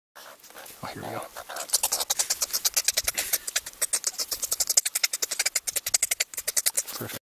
Polo is ticklish
Polo the meerkat has a very ticklish belly.